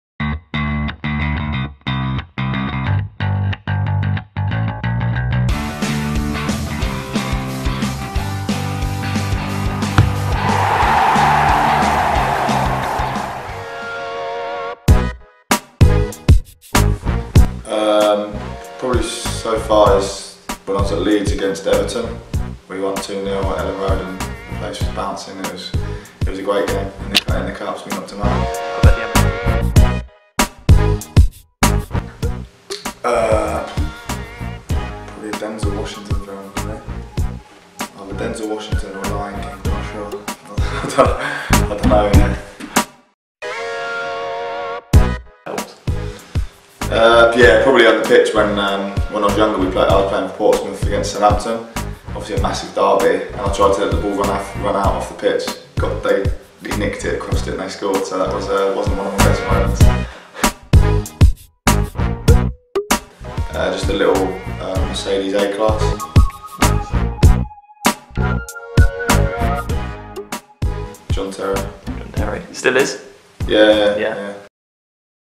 Quick-Fire Round: Jason Pearce
Wigan Athletic defender Jason Pearce is the latest professional footballer to take on Shoot's Quick-Fire Round.